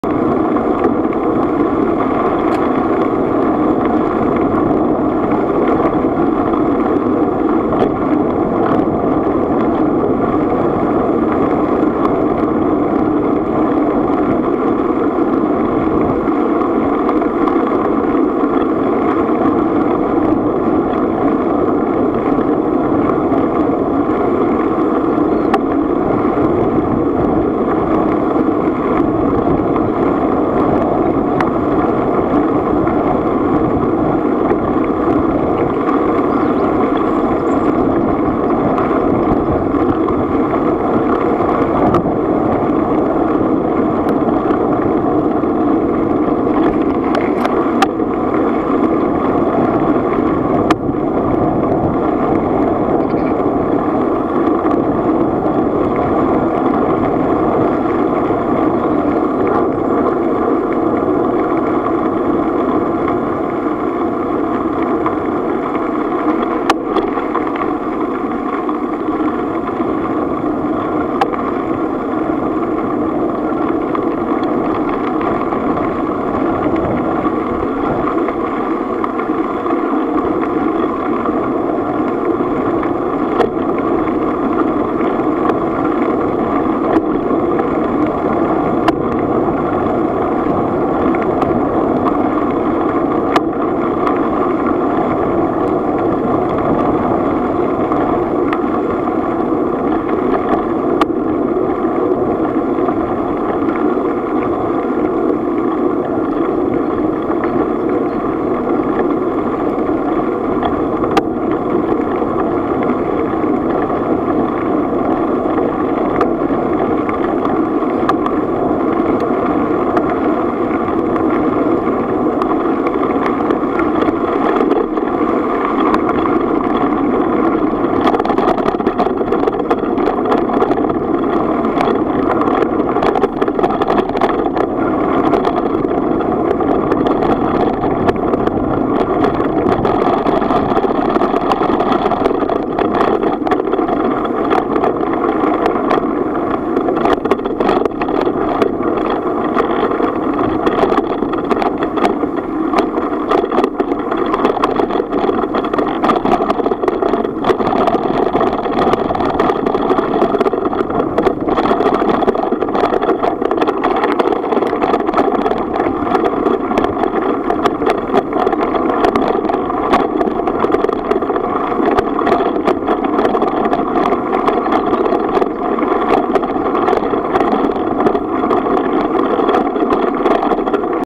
Stafford Greenway
The Stafford Greenway is a cycle and walkway along a disused railway line just a short distance from Stafford town centre, The pathway runs road free from Castlefields, Stafford all the way to Gnosall Heath only crossing one small country lane.